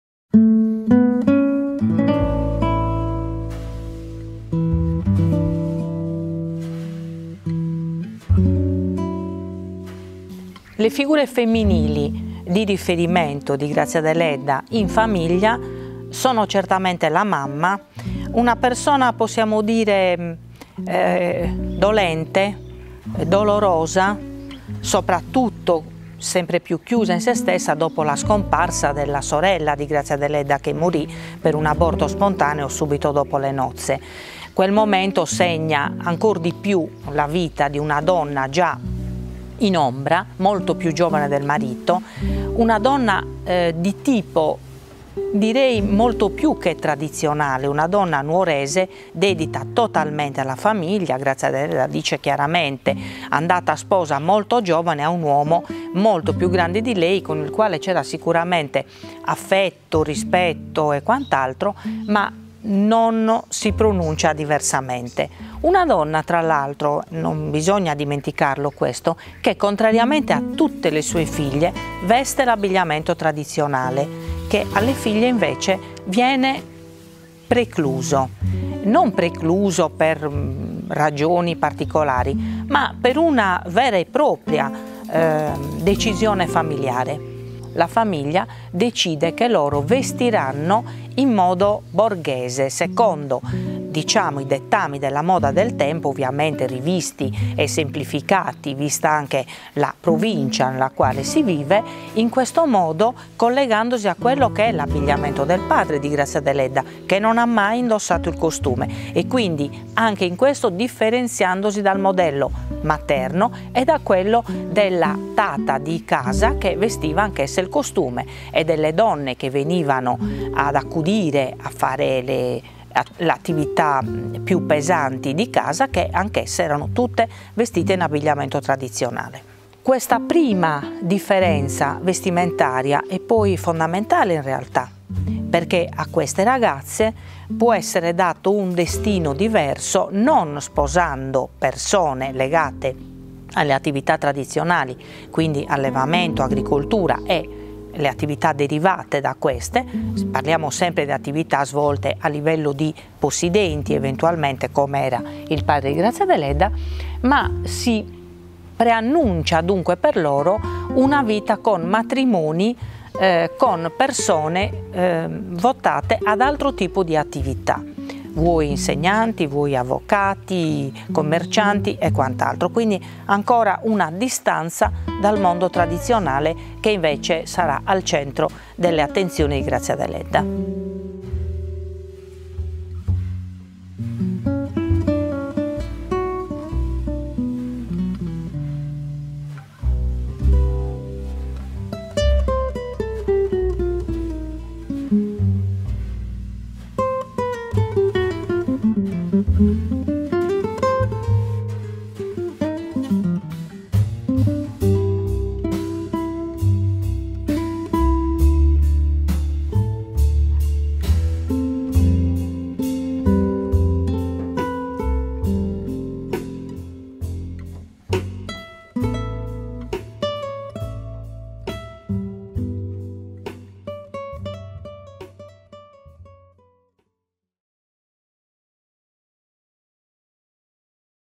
53. Franca Rosa Contu: Le decisioni prese dai genitori per Grazia e le sorelle
Grazia Deledda vista da vicino (sezione seconda: studiare Grazia Deledda)
Interventi: Franca Rosa Contu
Produzione esecutiva e coordinamento: Fabio Pillonca (Domus de Janas)
Regia e montaggio: Marco Gallus
Si ringrazia l’ISRE di Nuoro per la concessione degli spazi della casa Museo dedicata a Grazia Deledda